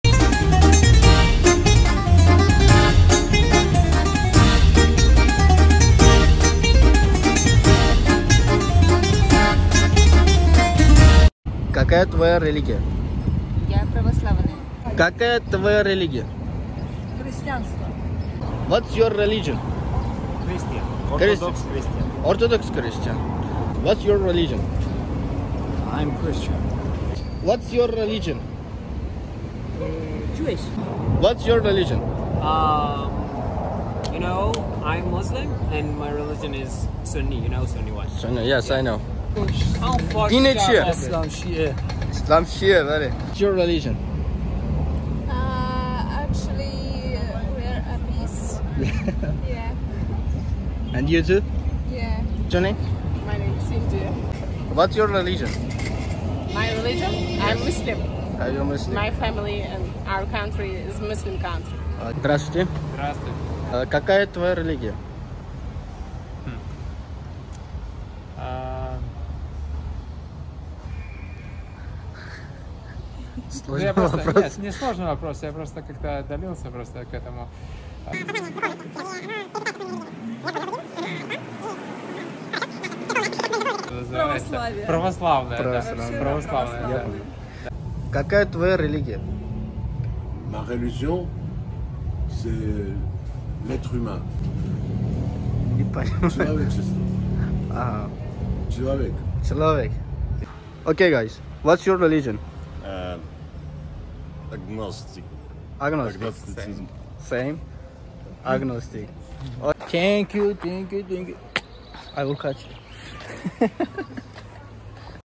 What’s Your RELIGION? (I don’t believe in God)… ~4
Hello everyone,
In this video I asked people What’s Your RELIGION ? - Georgia 🇬🇪